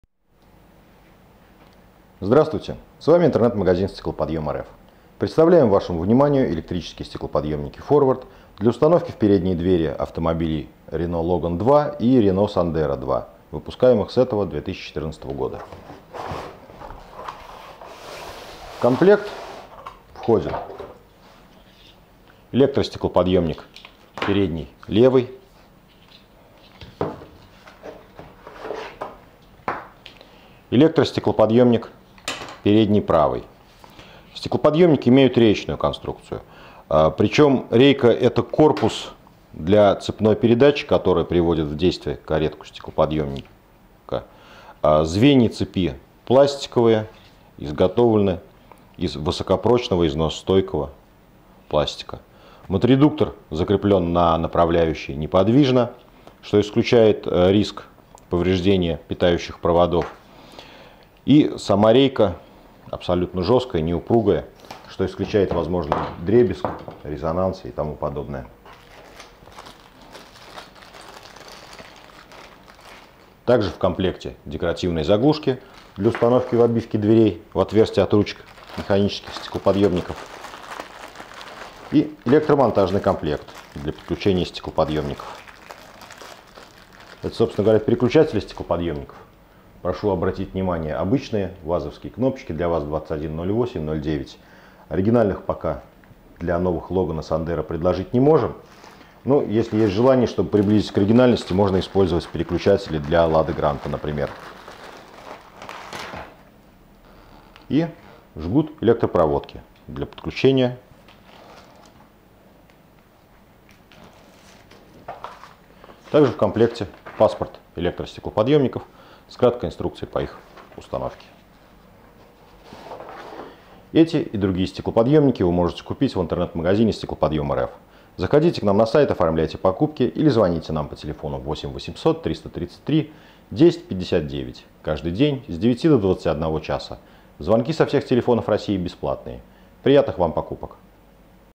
Электростеклоподъемники ФОРВАРД для Renault Logan II и Sandero II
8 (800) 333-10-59 - наш телефон для бесплатных звонков со всей России

Электрические стеклоподъемники в передние двери автомобилей Renault Logan II и Renault Sandero II, выпускаемых с 2014 года.
Комплект электростеклоподъемников с моторедукторами на две двери. В комплекте 2 стеклоподъемника, электропроводка для подключения, переключатели стеклоподъемников (92.3709, 2 шт.), декоративные заглушки в отверстия от ручек механических стеклоподъемников.
Рейка электростеклоподъемника является корпусом, в котором расположен механизм перемещения стекла — зубчатая цепная передача. На рейке неподвижно закреплен электродвигатель, приводящий механизм в действие.
Неподвижно расположенный моторедуктор исключает возможность повреждения электропроводки в двери.
Рейка стеклоподъемника абсолютно жесткая. Благодаря этому при подъеме и опускании стекла отсутствует дребезг, вибрация и резонансы.
Стеклоподъемники чрезвычайно легкие и компактные. Работают практически бесшумно.
Устанавливаются на места направляющих штатных стеклоподъемников и крепятся в штатные крепежные отверстия крепежными деталями заменяемых стеклоподъемников.